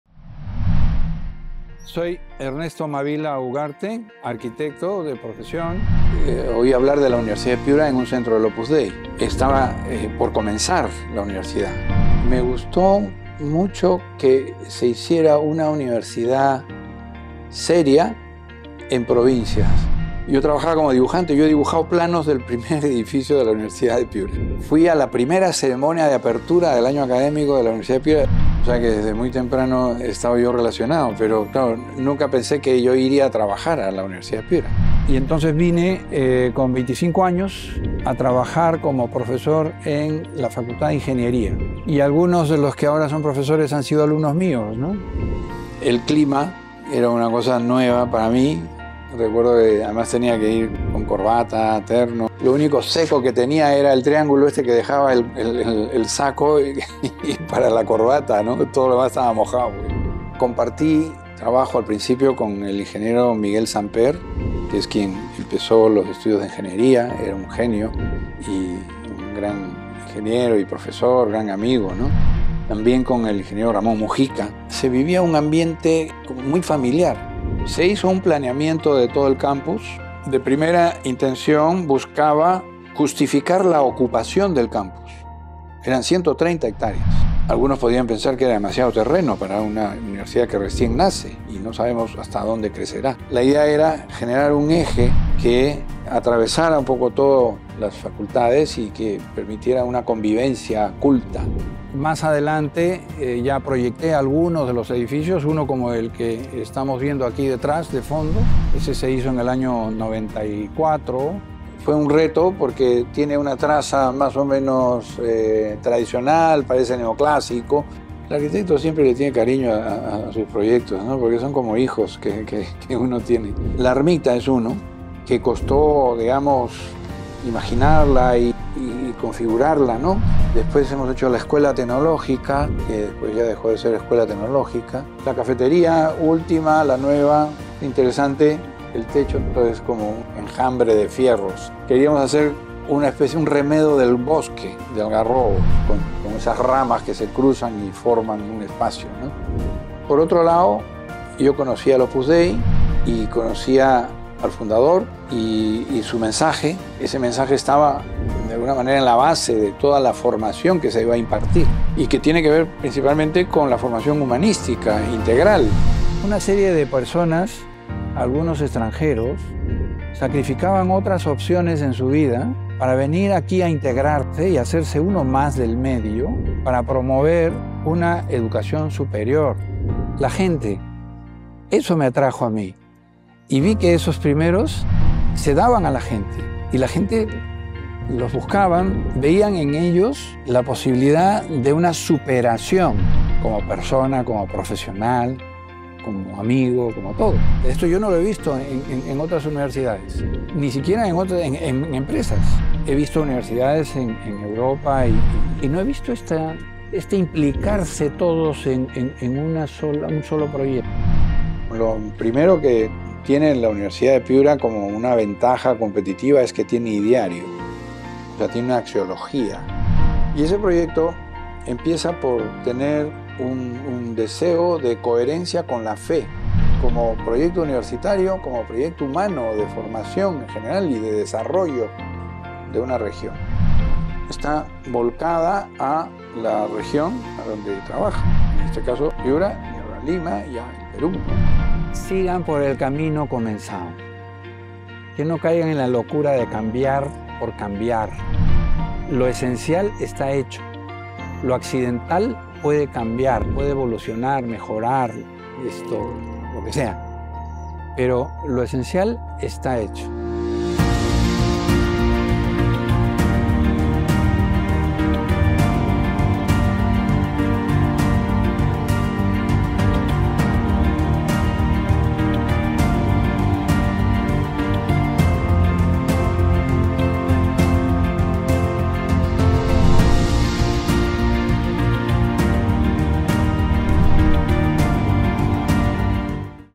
RostrosUDEP. Arquitecto Ernesto Mavila
RostrosUDEP. Conoce más de la trayectoria en la Universidad de Piura del arquitecto Ernesto Mavila, quien fuera vicerrector de la UDEP y decano de la Facultad de Humanidades. También fue el encargado de diseñar el edificio de Gobierno, el de Educación, de la Facultad de Comunicación, la Ermita, la nueva cafetería, entre otros espacios importantes del campus.